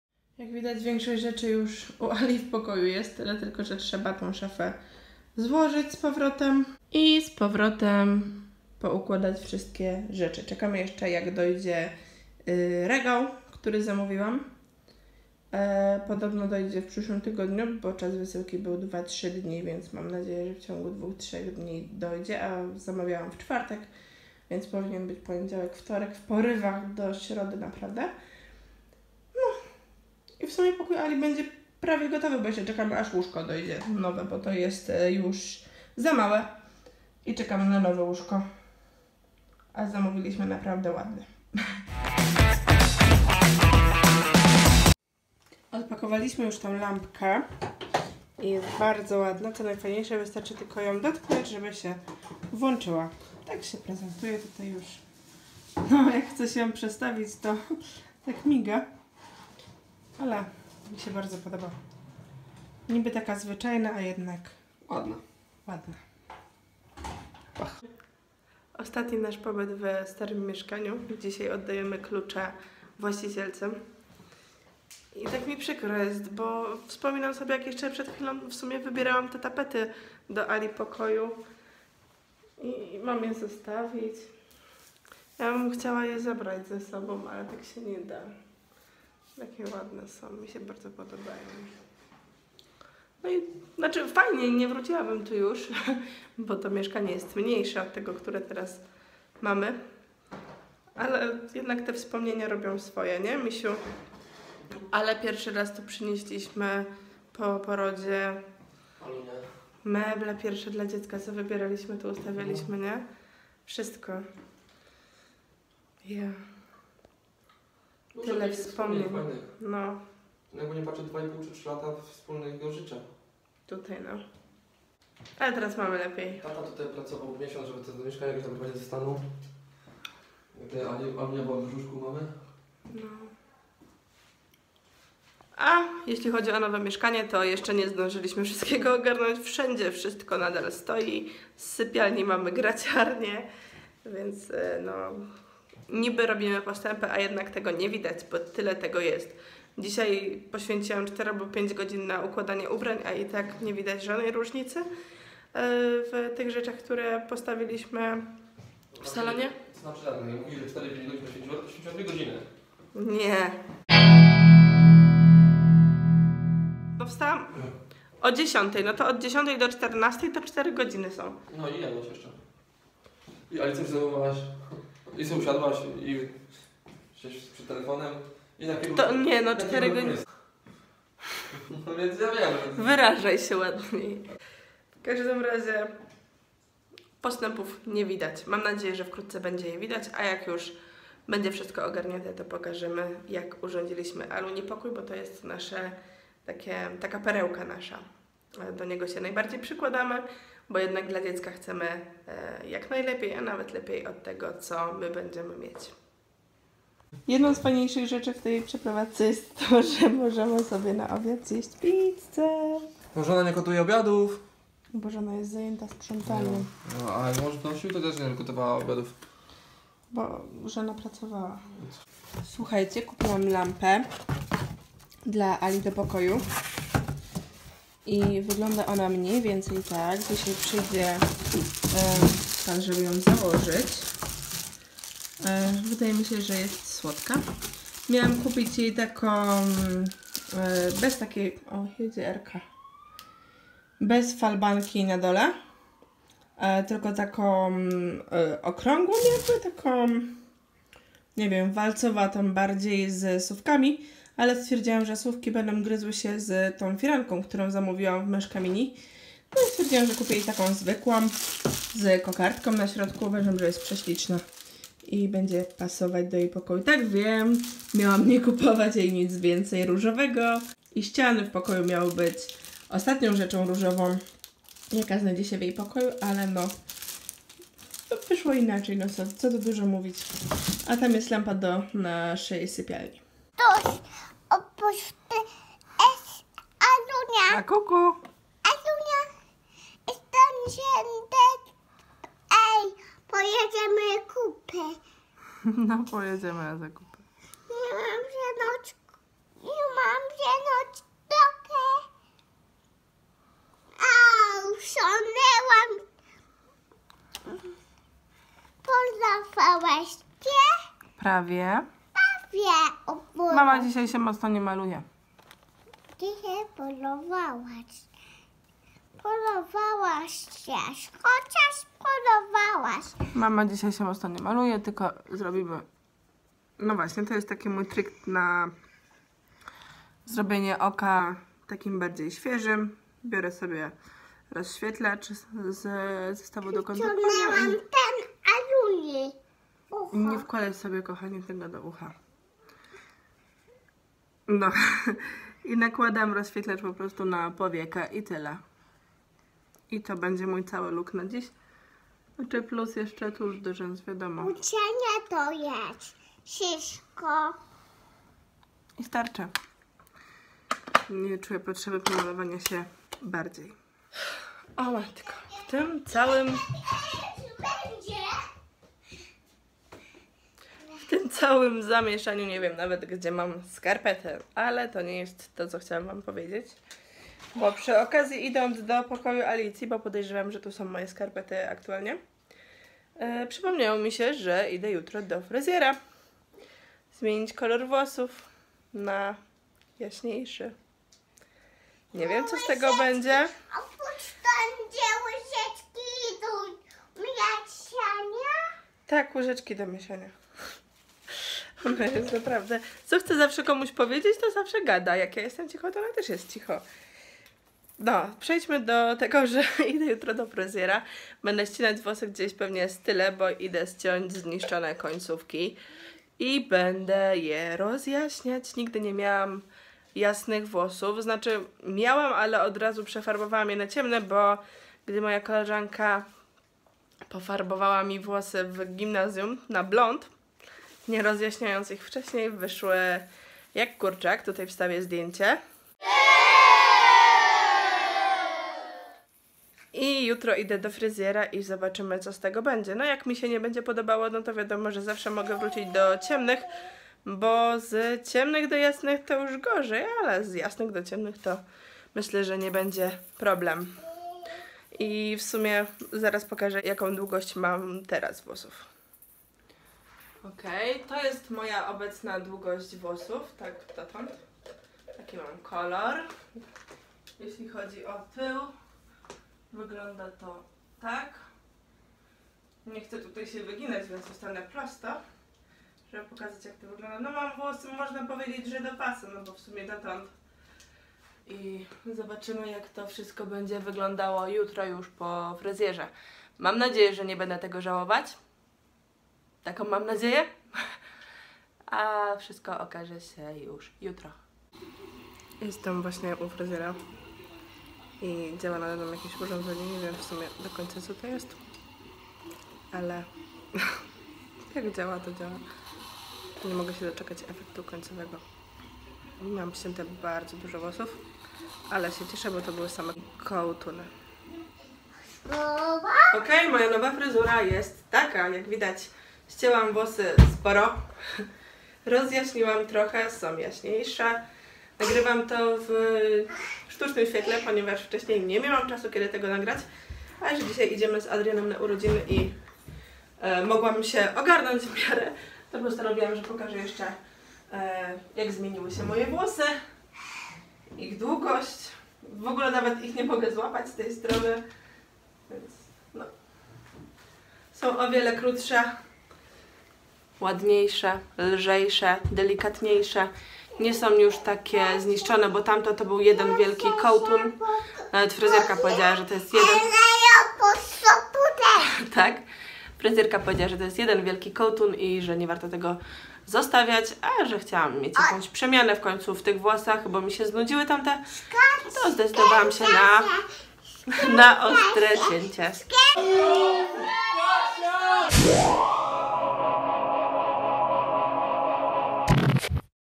PRZEPROWADZKA CZĘŚĆ 2. | ZwyczajnaMama
Część druga przeprowadzki, wizyta u fryzjera i impreza urodzinowa Kasi.
Czas przeprowadzki jest ciężki więc postanowiliśmy go sobie trochę umilić ;)